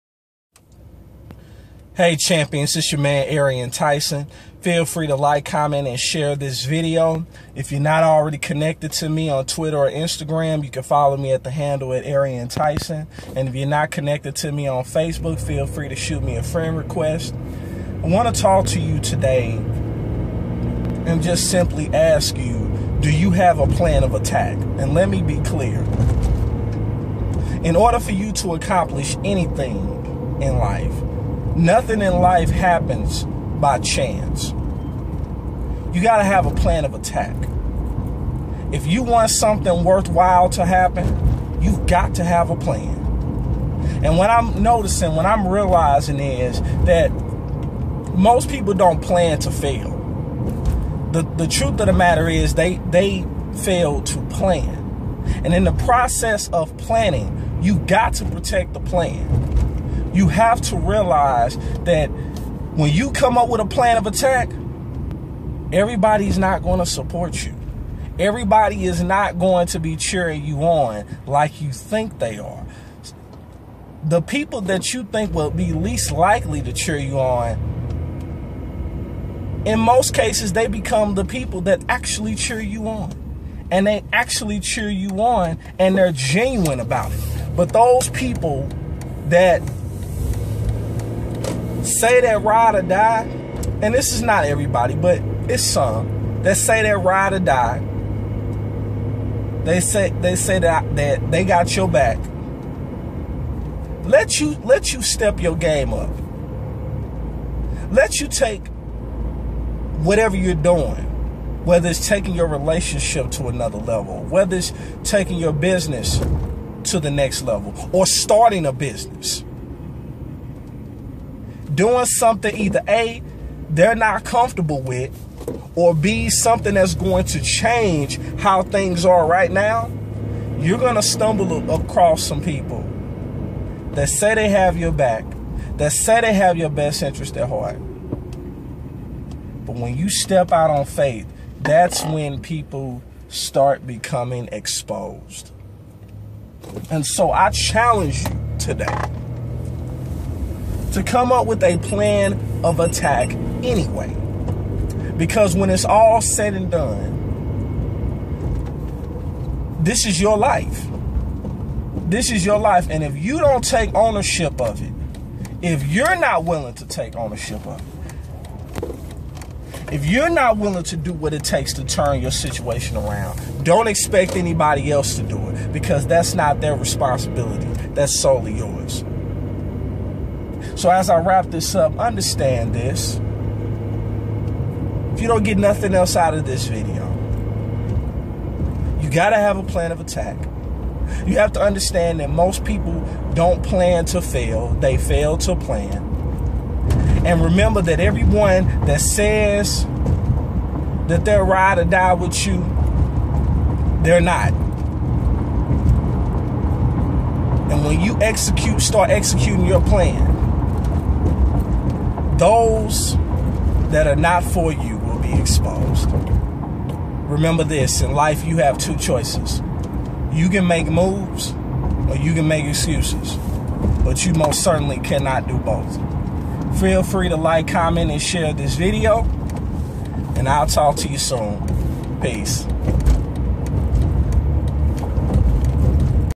What Is Your Plan Of Attack?
Nothing will change until you take action. What is your plan of attack? Most people don't plan to fail, they fail to plan. What actions do you need to take?